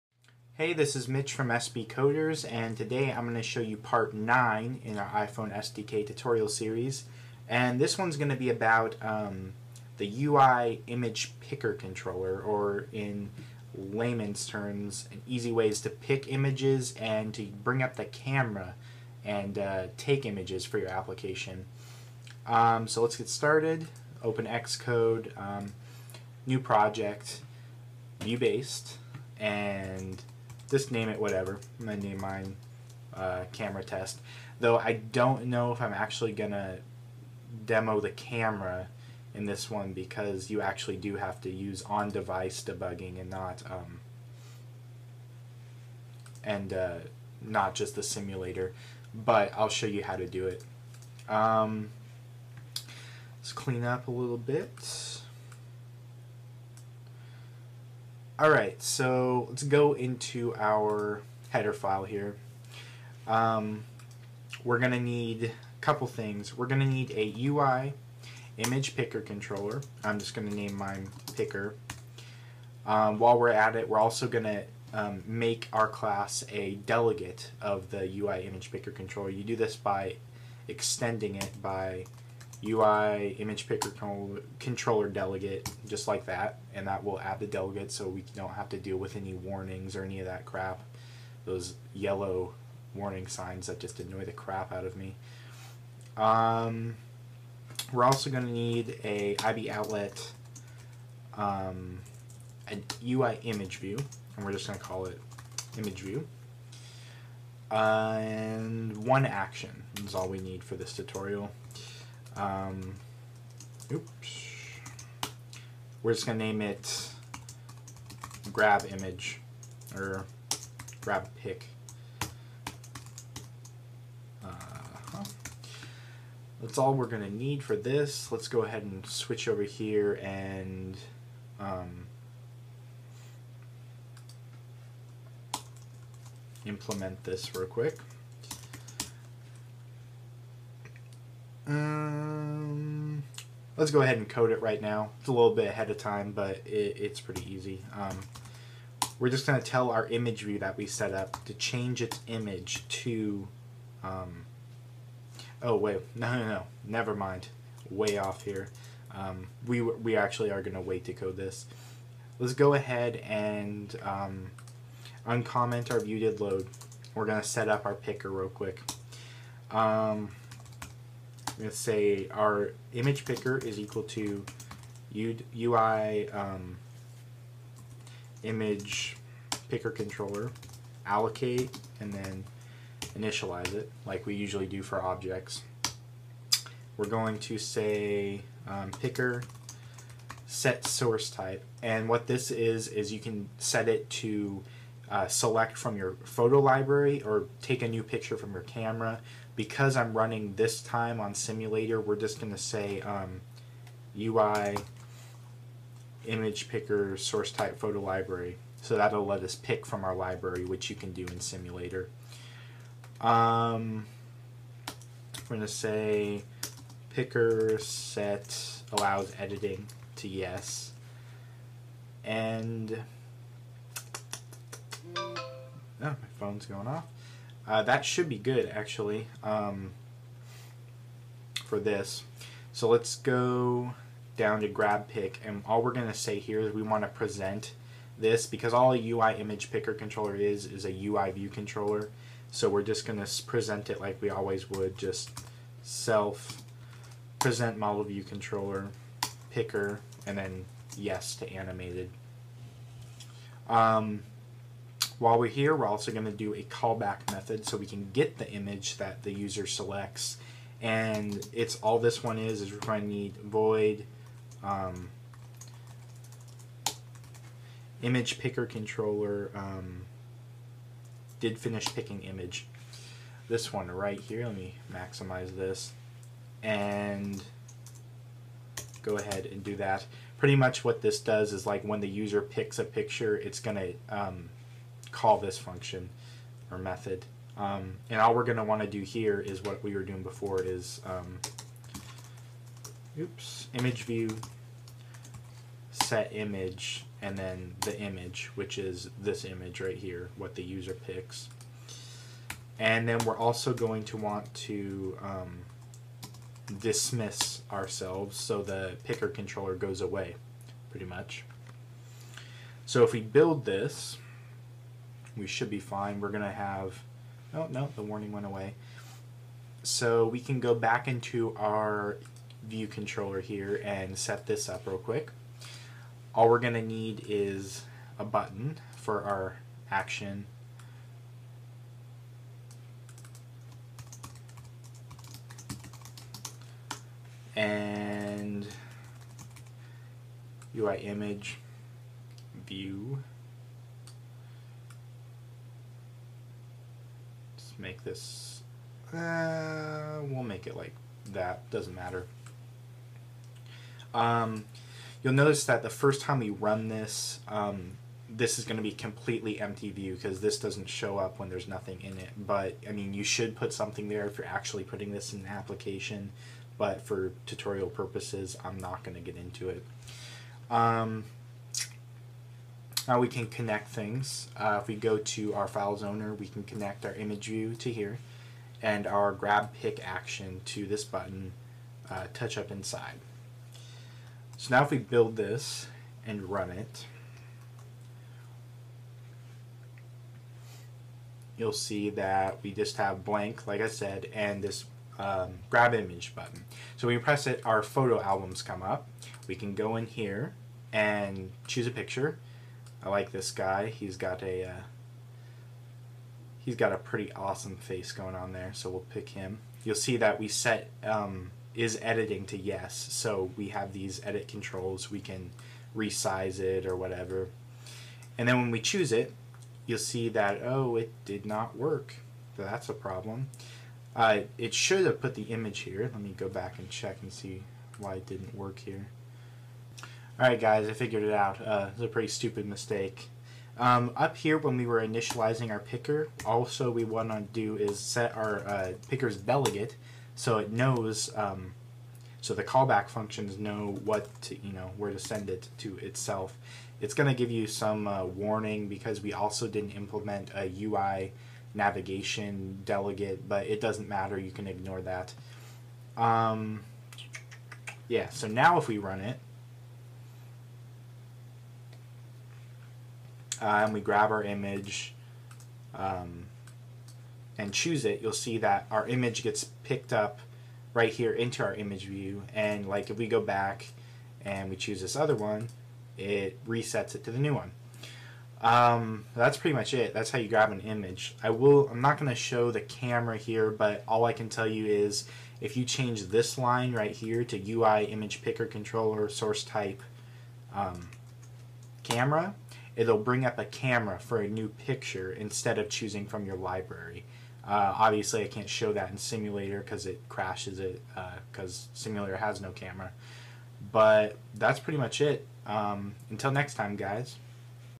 iPhone SDK Tutorial Series 9: UIImagePickerController
In part 9 of our iPhone SDK Tutorial Series, I will explain how to access the photo library, in order to select saved Images. I will also explain how to take pictures from the iPhone's camera from within your app.